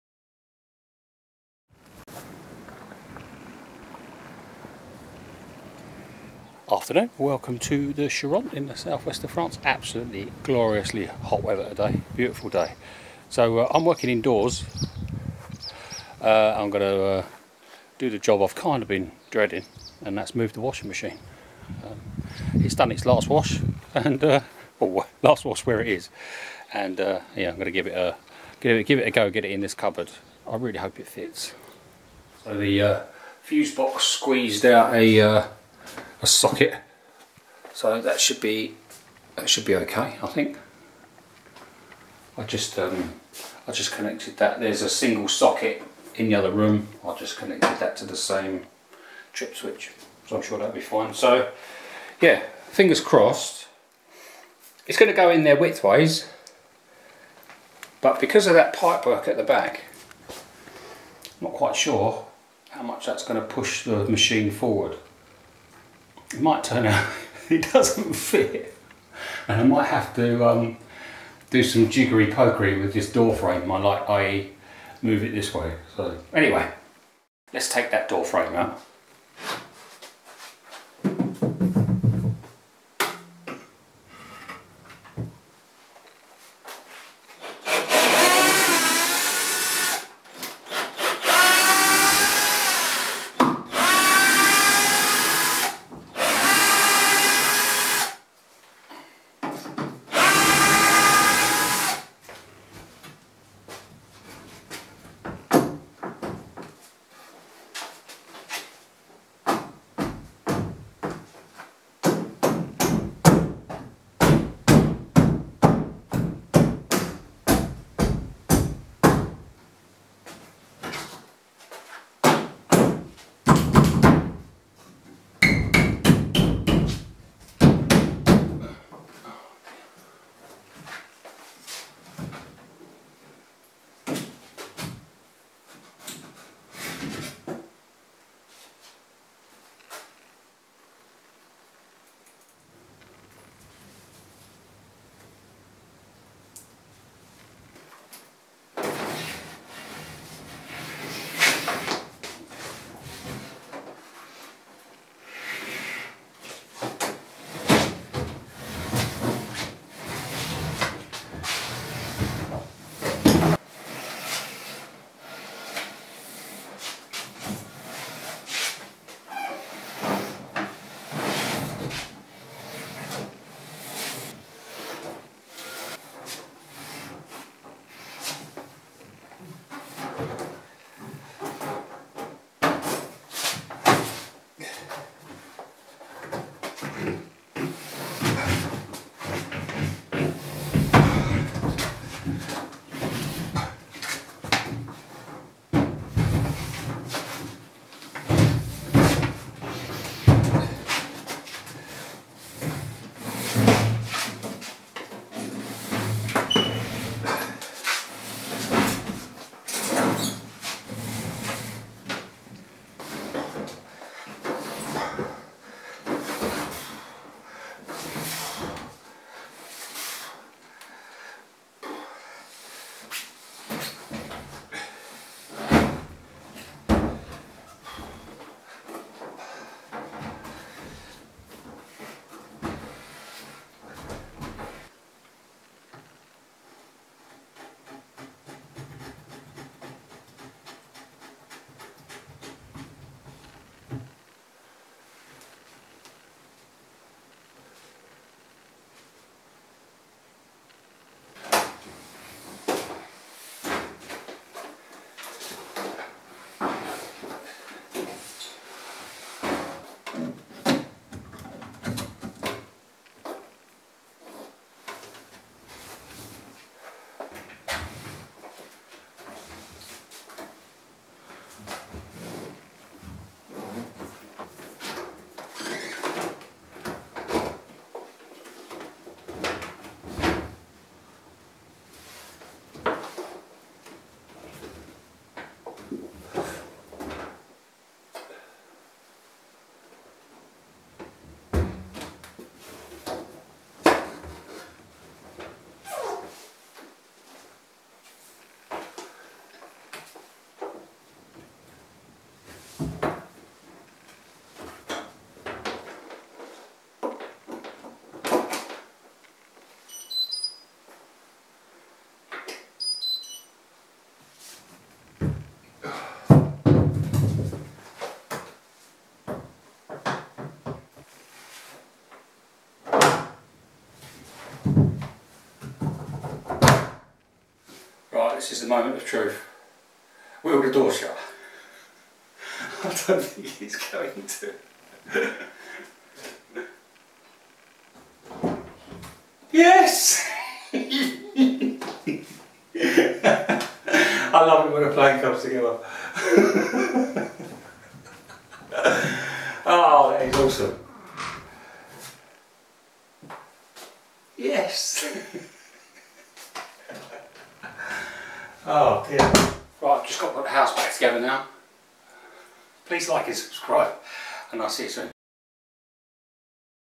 Will the washing machine fit in the cupboard? - My French homestead renovation
A job I haven't been looking forward to.